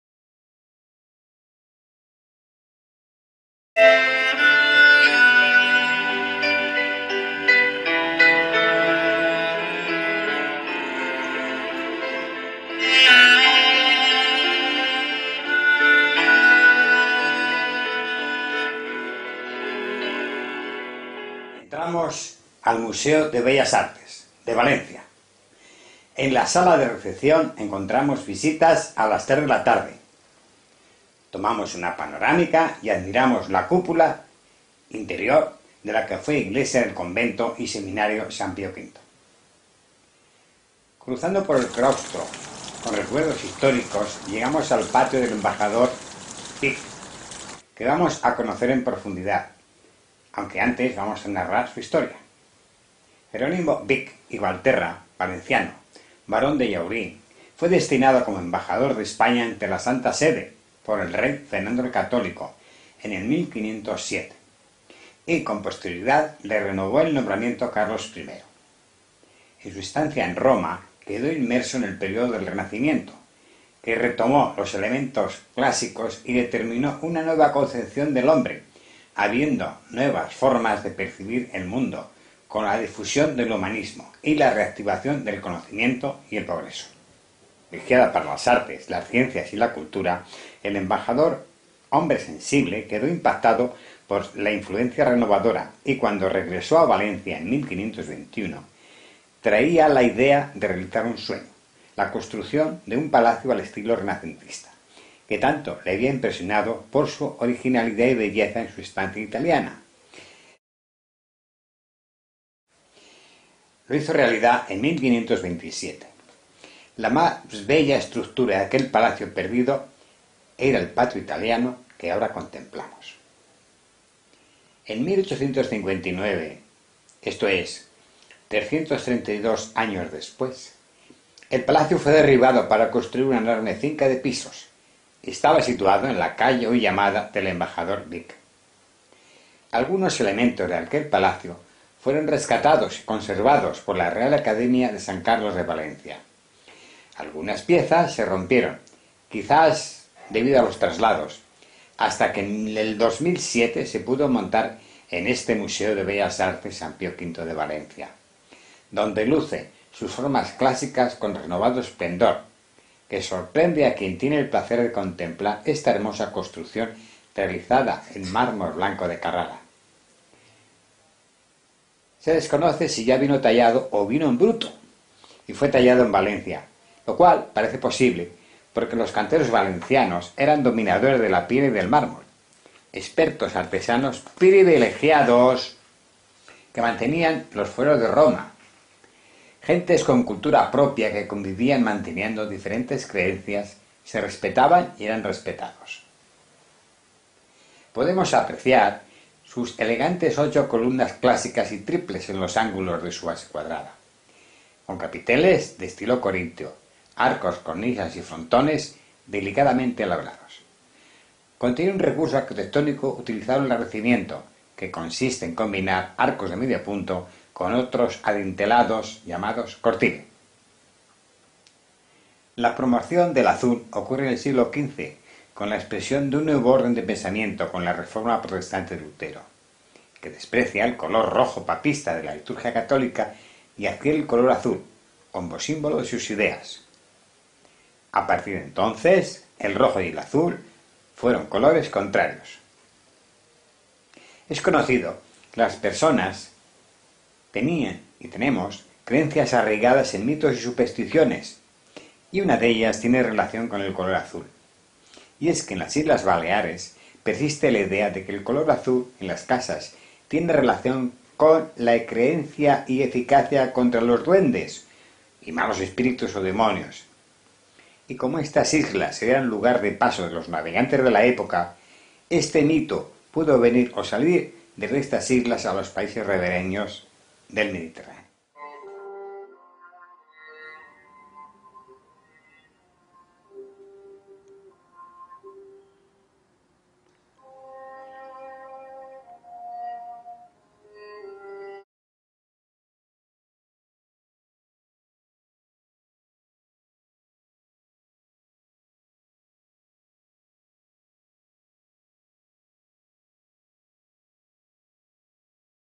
EL PATIO MEDIEVAL DEL EMBAJADOR VICH
SITUADO EN EL MUSEO DE ARTE SAN PIO V EN VALENCIA